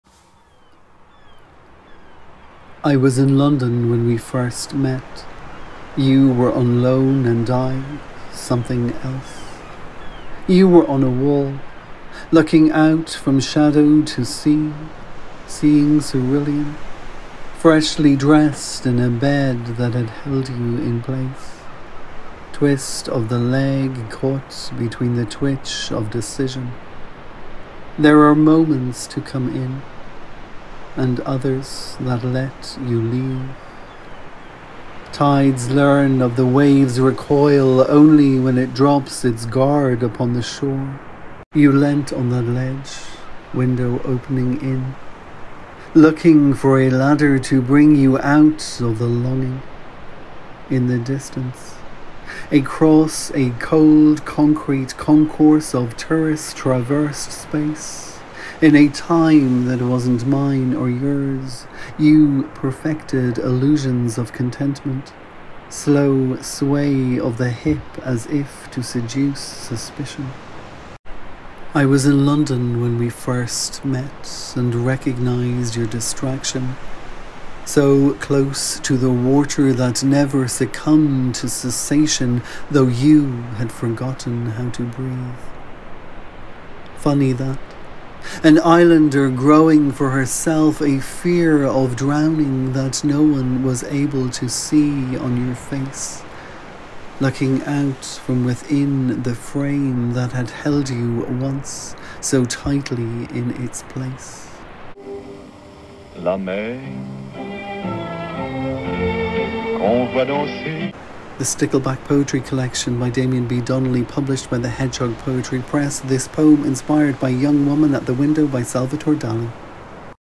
Dali Distractions - a short film poem
A short film poem inspired by the painting Young Woman at the Window by Salvador Dali from my Stickleback Poetry Collection published by The Hedgehog Poetry Press